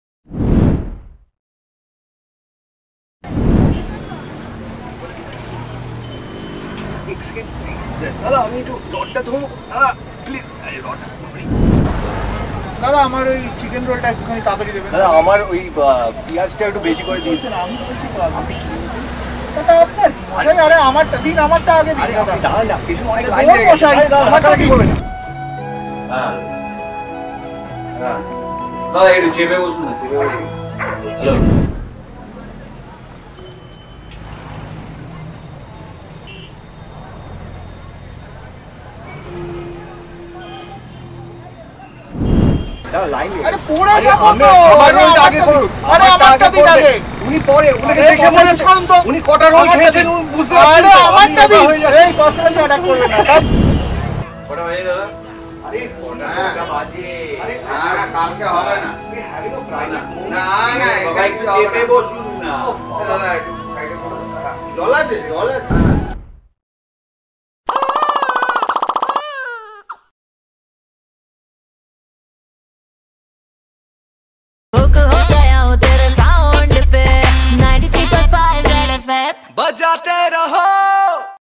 POPULATION KE SIDE-EFFECT II RJ AYAN
On World Population Day Rj Ayan shares his observations on everyday population!  Which one do you relate the most?

Now Enjoy your favorite show on the Red FM India App.